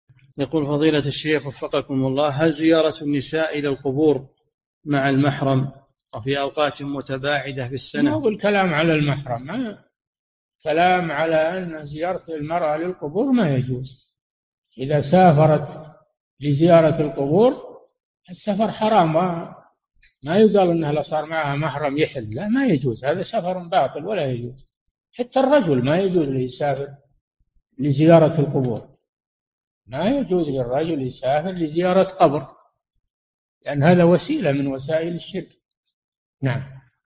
[165 -521] هل يجوز للنساء زيارة القبور مع المحرم؟ - الشيخ صالح الفوزان
-[165 -521] السؤال: هل زيارة النساء للقبور مع المحرم وفي أوقاتٍ متباعدة في السنة...؟
الجواب: ما هو الكلام على المحرَم الكلام على أن زيارة المرأة للقبور لا يجوز، إذا سافرت لزيارة القبور فالسفر حرام ما يُقال: إنها إذا صار معها محرَم يحل لا ما يجوز، هذا سفرٌ باطل ولا يجوز، حتى الرجل ما يجوز له يُسافر لزيارة القبور، ما يجوز للرجل يُسافر لزيارة قبر، لأن هذا وسيلة من وسائل الشرك.
للاشتراك في قناة الشيخ صالح الفوزان

للاستفسار والإقتراحات والدعم
رئيس الفريق
لمشروع كبار العلماء